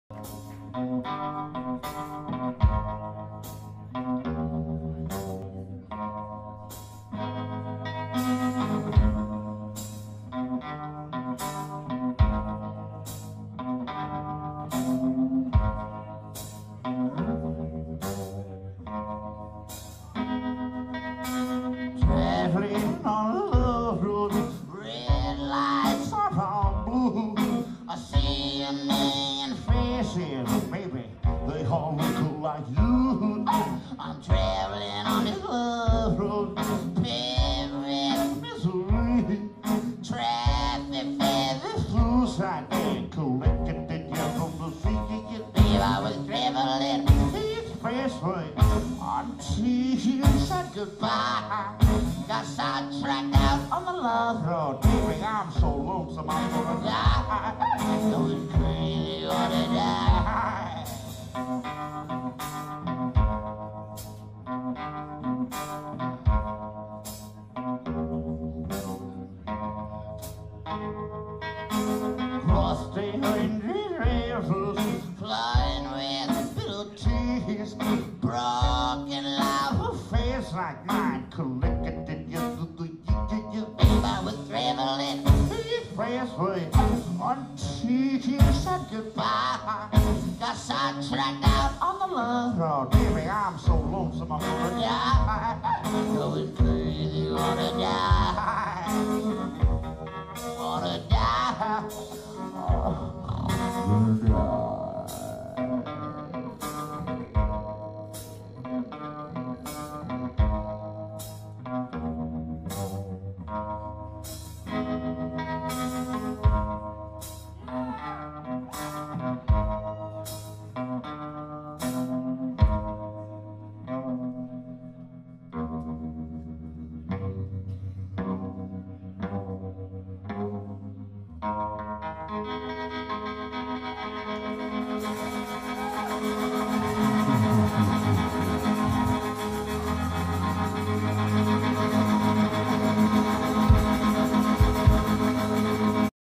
Bloodshot Bill 02
Live performance by the one man band Bloodshot Bill. Sporting pajamas, a smokers jacket bare feet and a comb, this was a terrific show. Go see him in concert, buy his many different CDs, vinyl and other product. Recorded at Middle East Downstairs in Boston, fall 2012